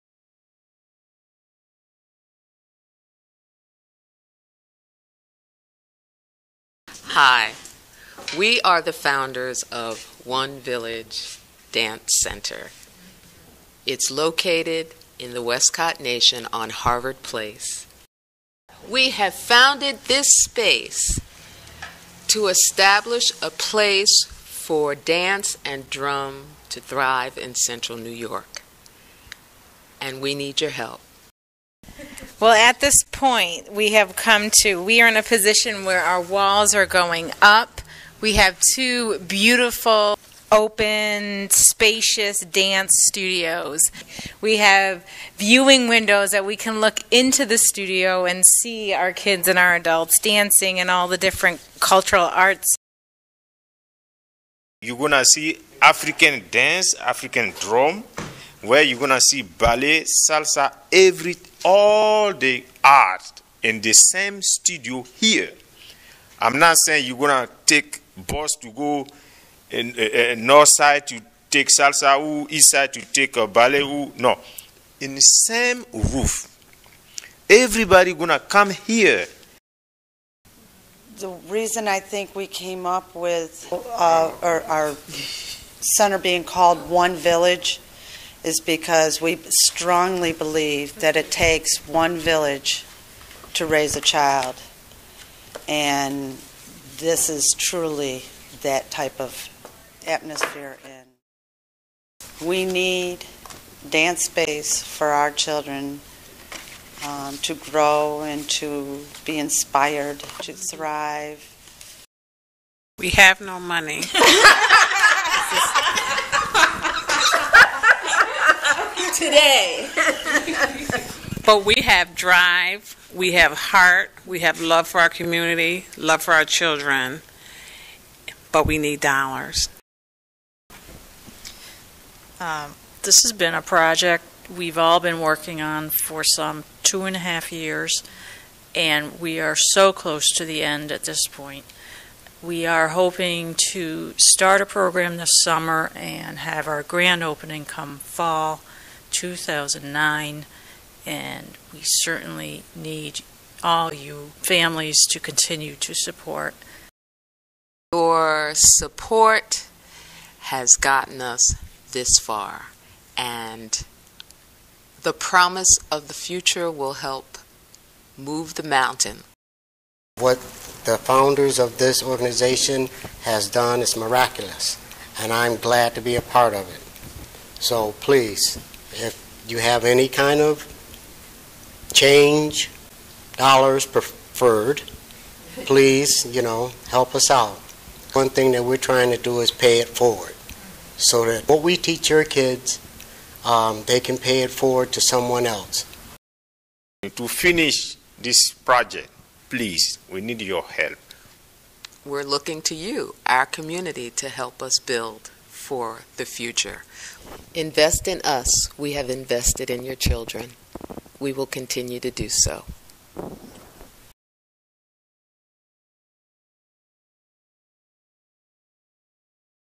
One Village Video (Final)
PennyMaria Jackson produced this multimedia project for One Village Dance Center in Syracuse, New York.  As the company was near the end of it's building renovation process, some funding fell through.  This project was an outcry for more donations from local companies and individuals.